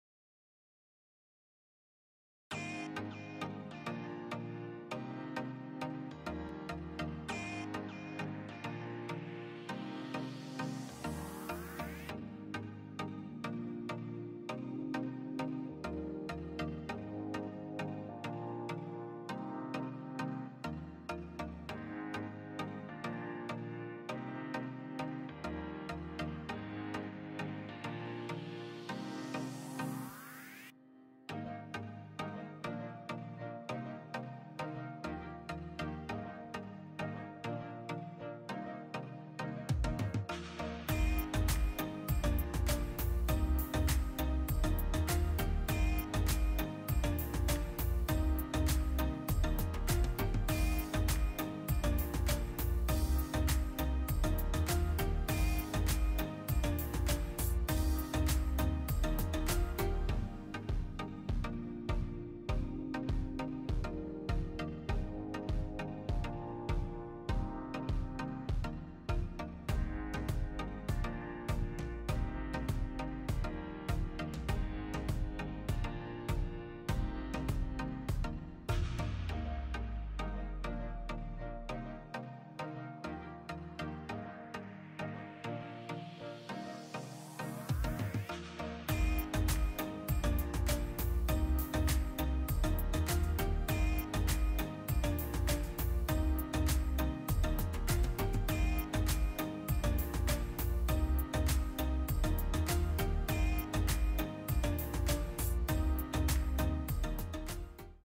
Fena: Pirate Princess - Japanese Anime Title in Japanese writing - How to write Pirate Princess
This is how you write Japanese Anime Fena: Pirate Princess in Japanese.
海賊(Pirate)
王女(Princess)
In this channel, I will be writing various Japanese words, mainly from anime and games.